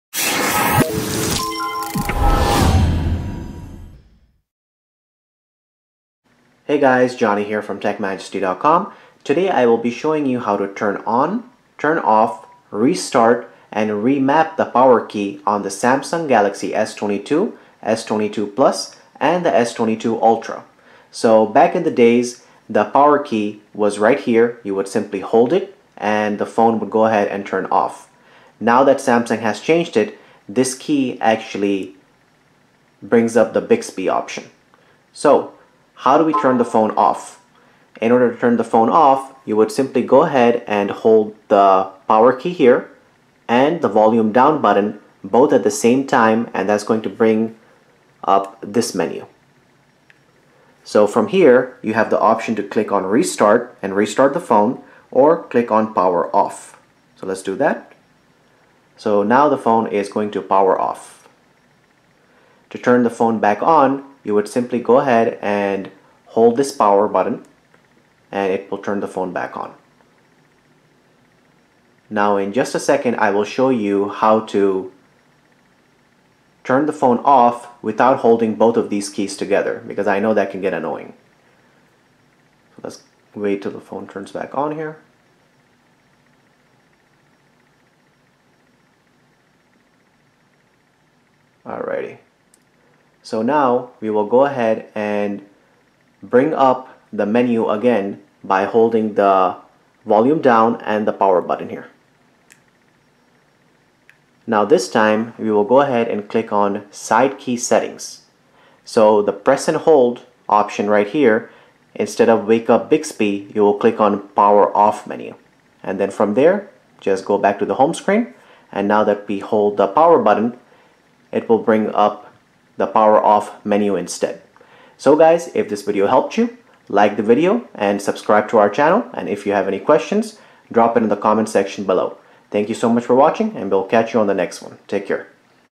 How to Turn Off/Turn On or Restart: Samsung Galaxy S22, S22+ (Plus), S22 Ultra - Remap Power Button
- In this video we will show you how to turn your Samsung Galaxy S22, S22+, and S22 Ultra OFF, how to turn it ON, how to restart it, and how to remap the power button.